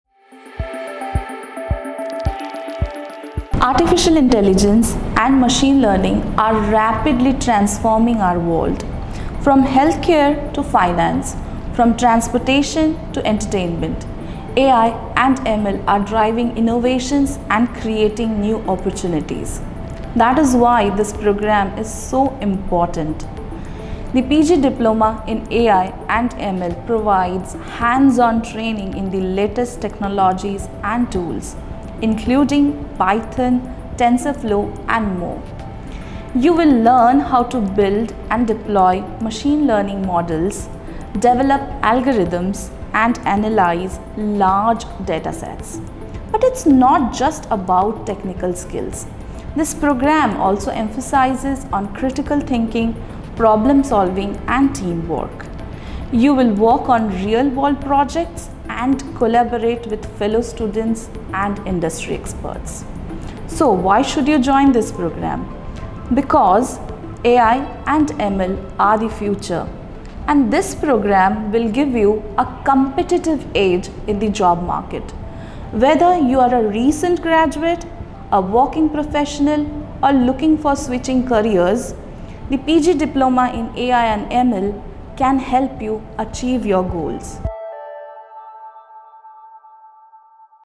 PG Diploma in Artificial Intelligence and Machine Learning
The AI-ML program covers essential topics like Statistics, Machine Learning, Deep Learning, Natural Language Processing, and Reinforcement Learning. Live sessions by global practitioners, labs, and industry projects are all incorporated into this program through our interactive learning model. Aims at acquiring industry-valued skills and the most commonly used tools and techniques.